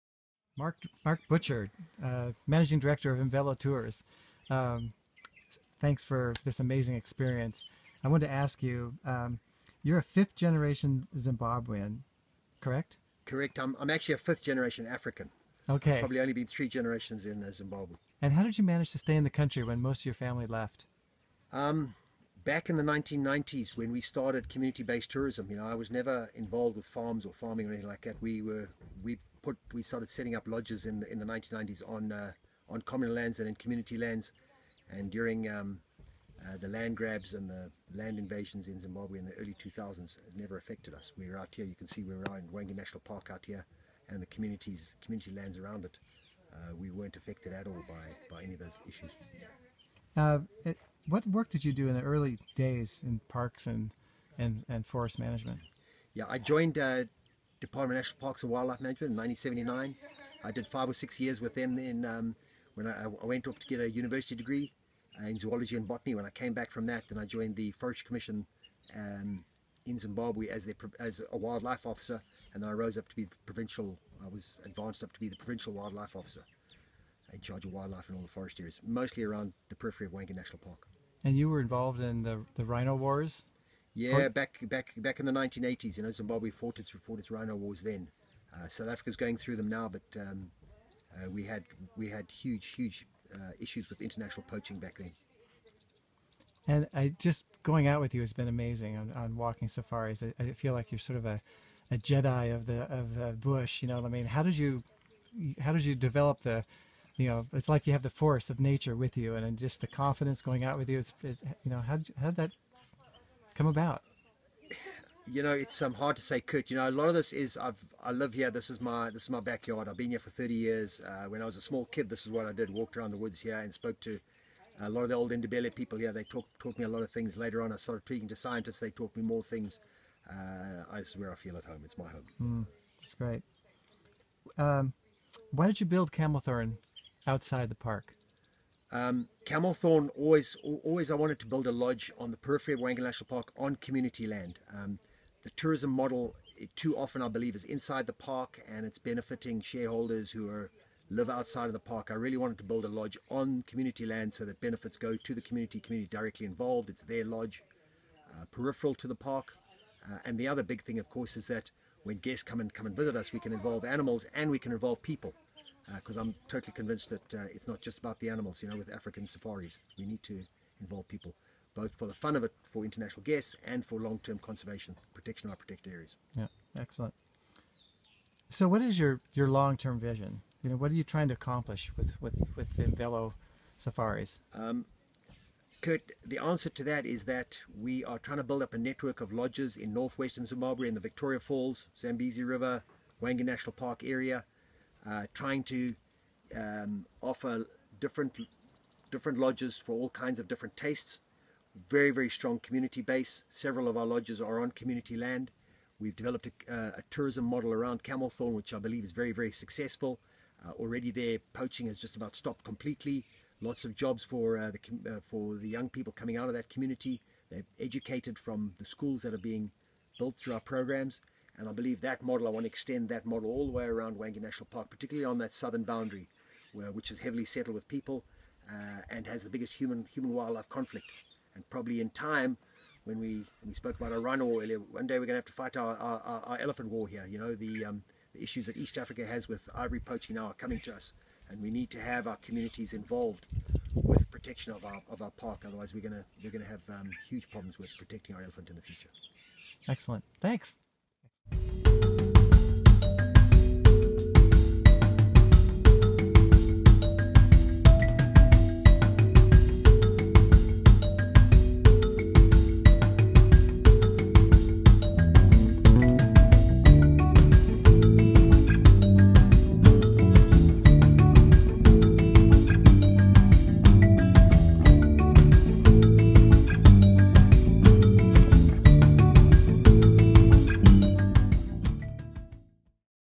Mark Butcher Interview in Zimbabwe
Interview with Imvelo Safaris Managing Director Mark Butcher by Wildland Adventures Founder Kurt Kutay.

Love travel tips?